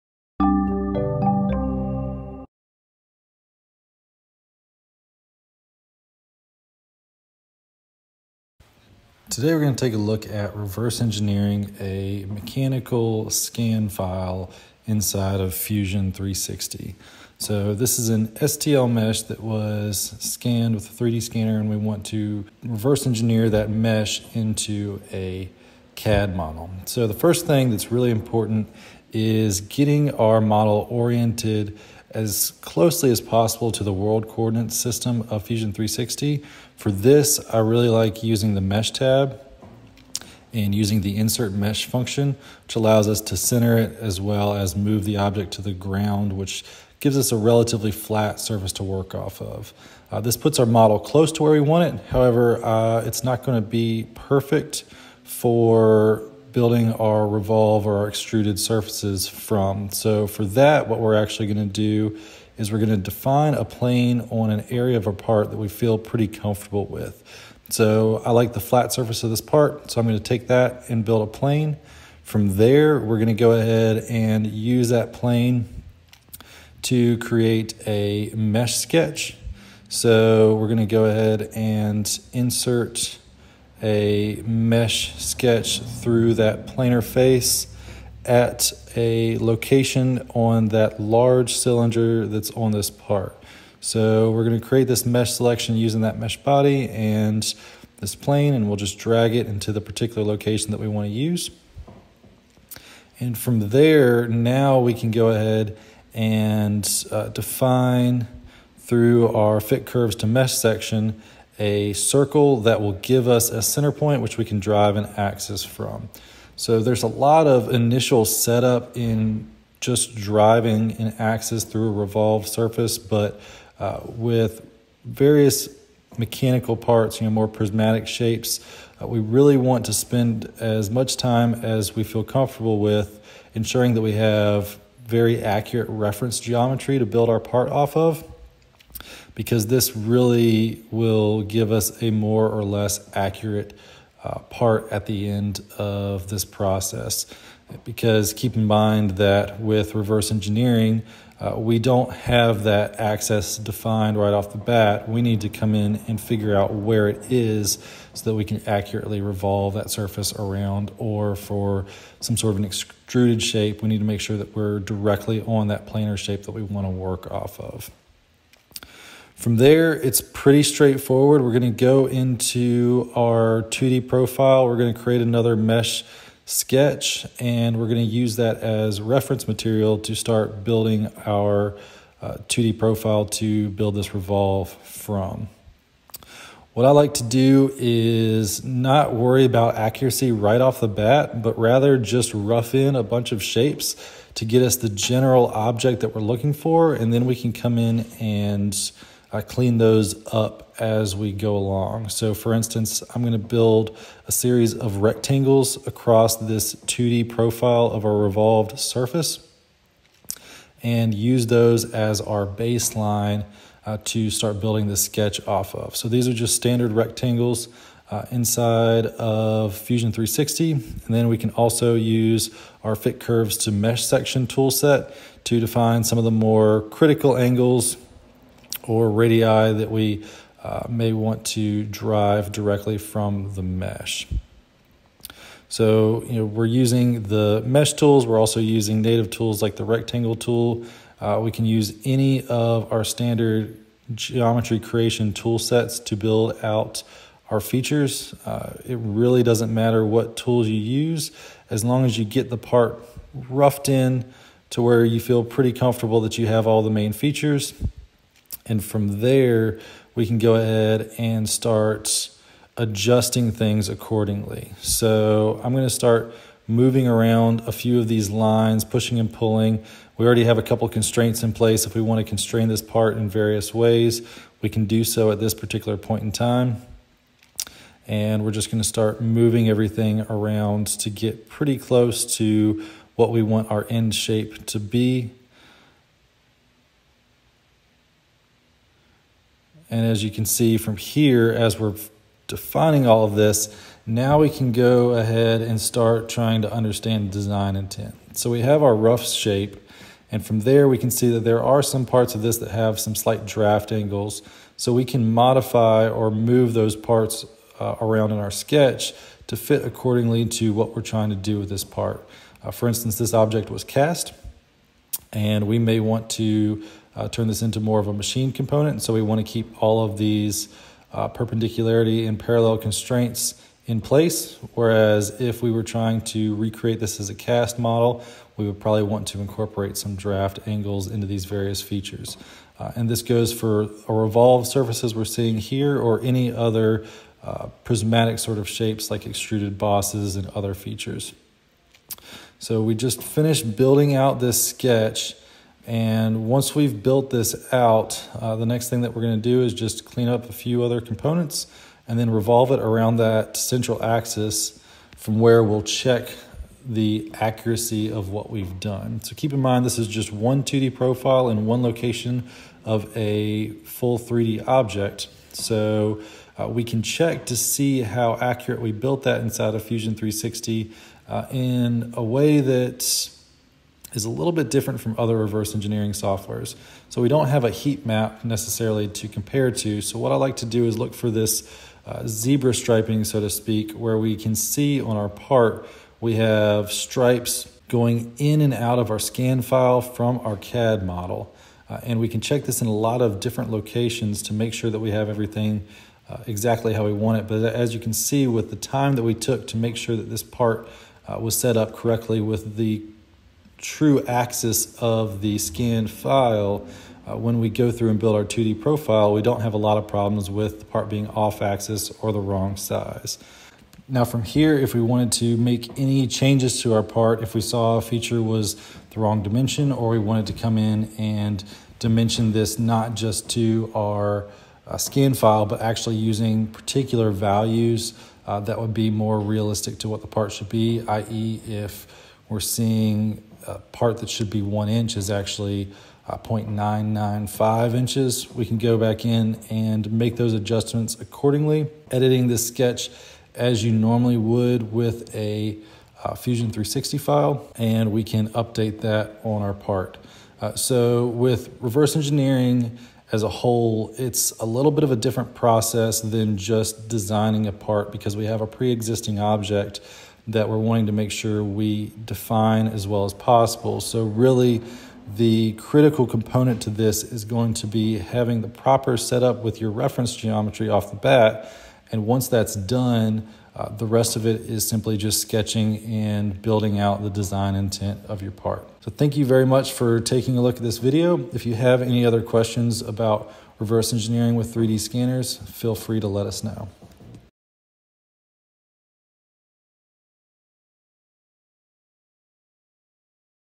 Reverse Engineering A Scanned Part in Autodesk Fusion 360 (STL to CAD)
When you need to design a part with precision, it's easier if you have a reference. In this video, we'll show you how to do that using STL mesh of a mechanical part to guide our new design inside Fusion 360.
Timestamps:
0:00 Introduction
0:26 Orient the part to the world coordinate system
3:00 Sketching 2D profile
5:50 Revolve the 2D profile into 3D
6:07 Check accuracy of sketch against 3D mesh for comparison
7:00 Make adjustments as needed and design intent